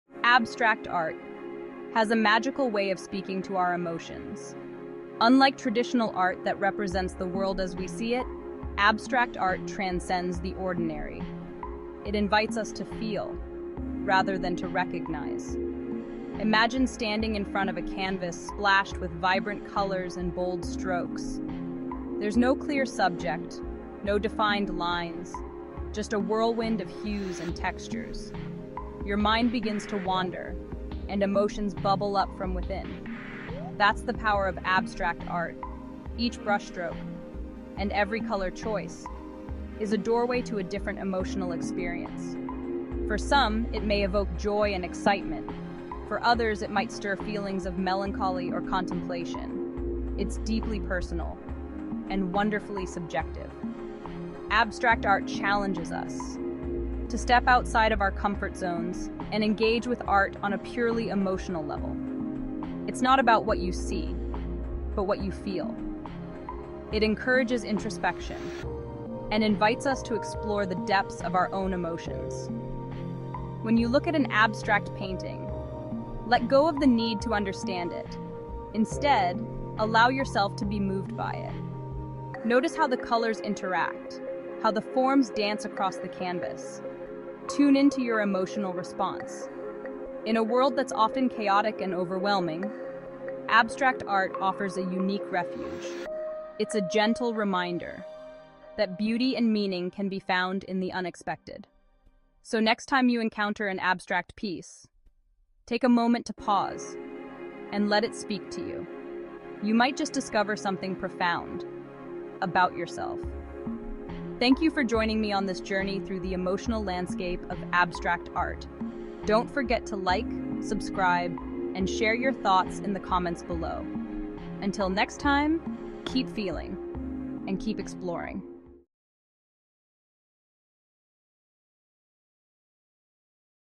Unlocking Emotions Trough Art
Dive into the mesmerizing world of abstract art! Discover how abstract art transcends traditional representations and invites a profound emotional engagement. Each brushstroke and color choice evokes unique, personal experiences, ranging from joy to melancholy. Engaging with abstract art not only encourages introspection but also allows for a deeper exploration of your own emotions. Join us as we delve into the beauty and complexity of this captivating art form.